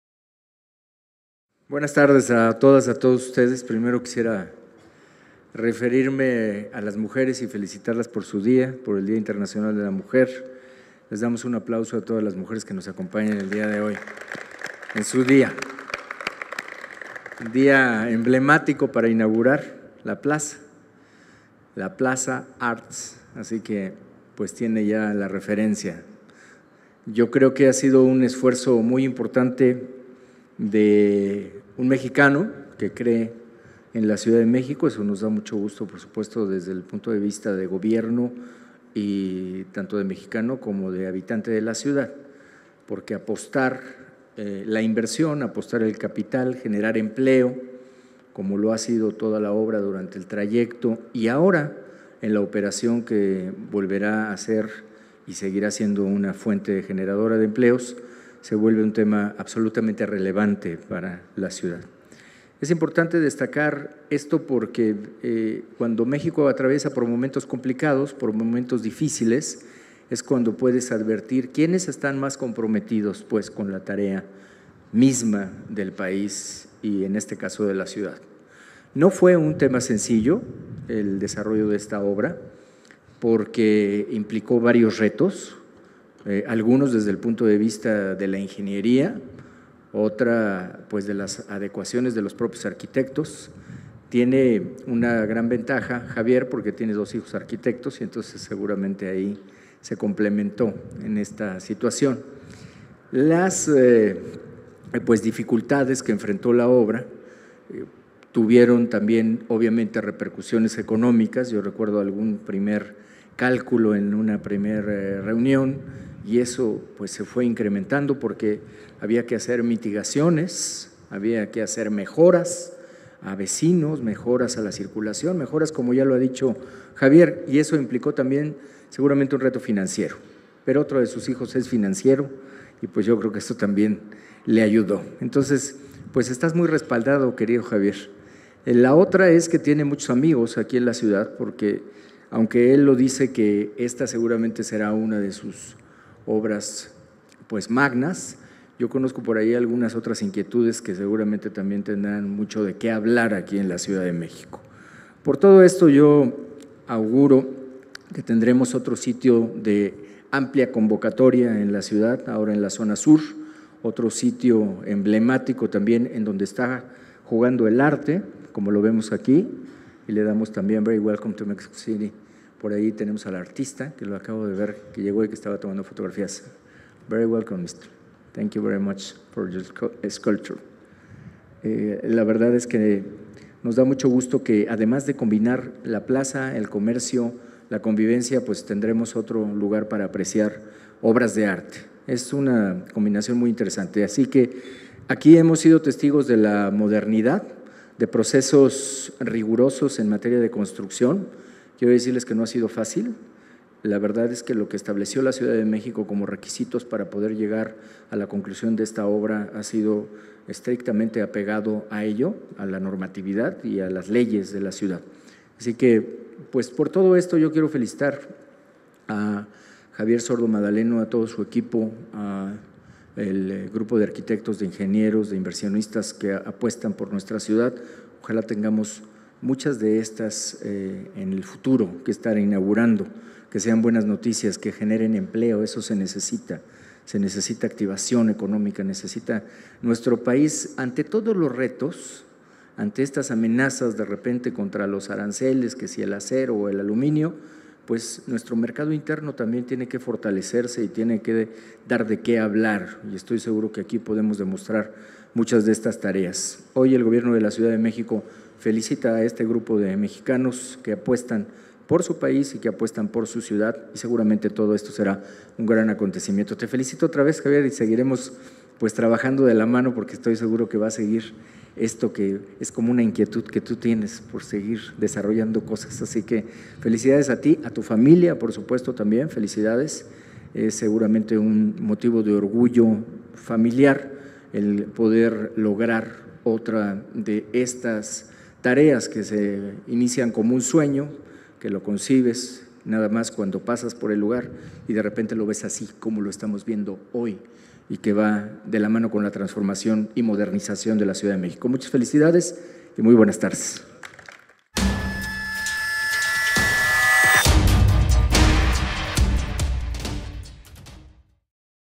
Así inauguró Miguel Ángel Mancera la Plaza Artz Pedregal
Fecha: 13/06/2018
Hora: 08:30
Lugar: CDMX
Tema: Corrupción
En la tarde de aquel jueves, Mancera Espinosa fue parte de la comitiva para celebrar la apertura de la plaza Artz. Estuvo acompañado del arquitecto Javier Sordo Madaleno, con quien realizó un recorrido por el recinto. Ambos caminaron por el patio del lugar y visitaron la zona de tiendas y locales.